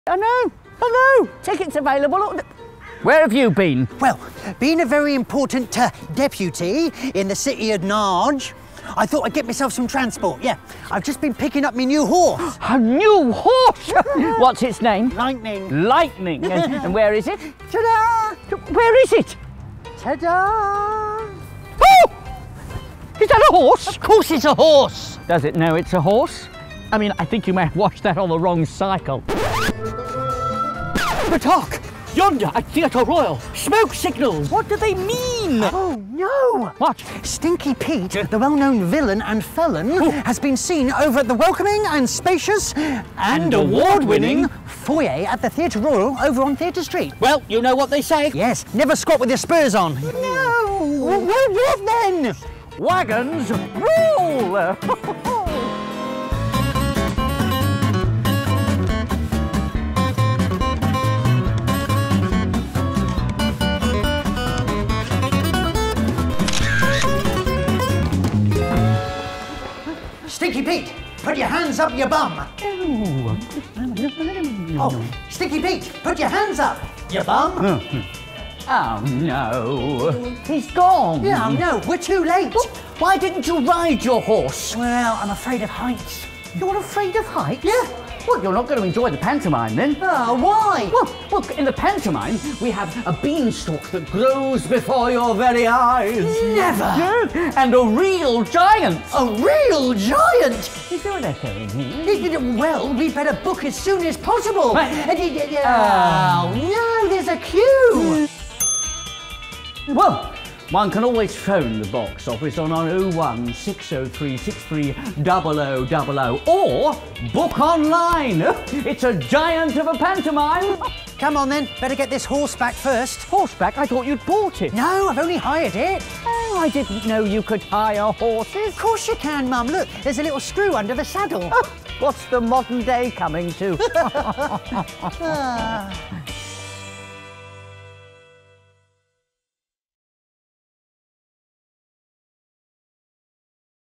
Jack and the Beanstalk trailer
Nigella Trottalot and her son Silly Billy are getting ready for Jack & the Beanstalk, our Christmas panto.

Down on his luck and barely able to afford the rent, Jack is tricked into selling the family cow for a handful of magic beans! On his return home Jack's Mum is furious and their madcap journey to the top of the beanstalk begins.

A giant of a magical adventure - climb on for Fee-Fi-Fo Fun!